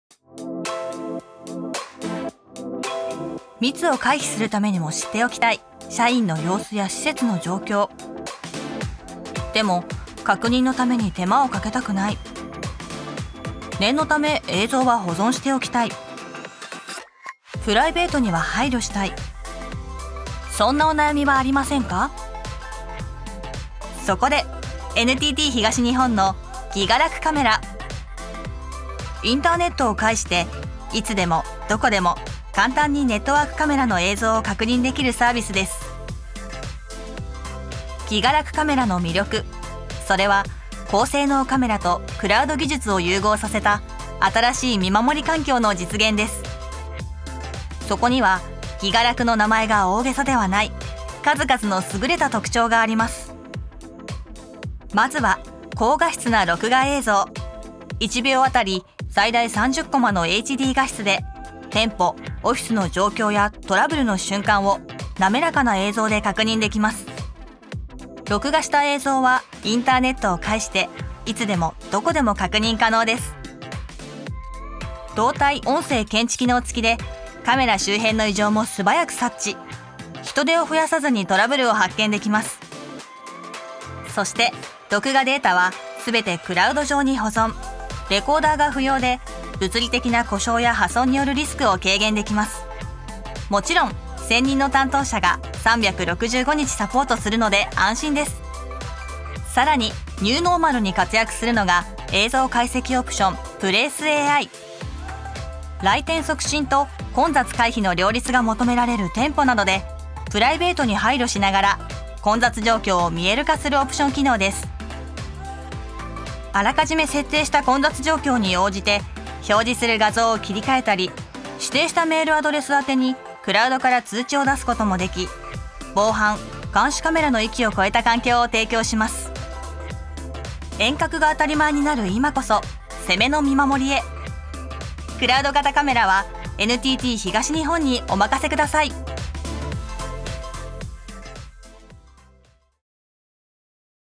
NTT東日本 いつでもどこでもオフィス・店舗の様子を確認できる！「ギガらくカメラ」サービス紹介
さあ、ニューノーマル。
見守りはどうする？

密を回避するためにも知っておきたい、社員の様子や施設の状況。

▼こんなお困りごとありませんか？
・確認のために手間をかけたくない
・念のため映像は保存しておきたい
・プライベートには配慮したい

そんな悩みをNTT東日本が提供するサービスで解決しませんか？

▼クラウド型カメラサービス「ギガらくカメラ」とは
インターネットを介して、いつでも、どこでも、簡単にネットワークカメラの映像を確認できるサービスです。

さまざまな問題をICTの力で解決をサポートいたします。
是非一度NTT東日本にご相談ください。